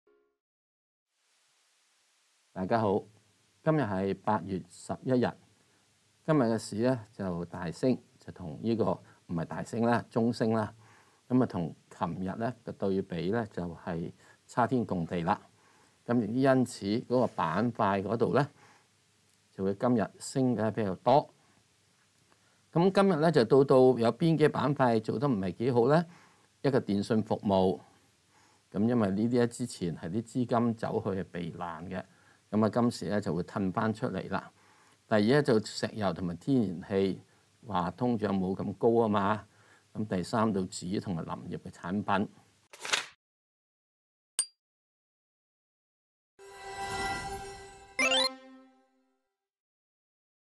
【石Sir午市閒談】今日推介中遠海控、康哲藥業 並分析受壓股藥明生物
恒生指數中午收報19959點，升348點。期指應如何部署？《香港經濟日報》專欄作者石鏡泉（石Sir）推介中遠海控（01919）、康哲藥業（00867），並分析受壓股藥明生物（02269），詳情待石Sir為你分析。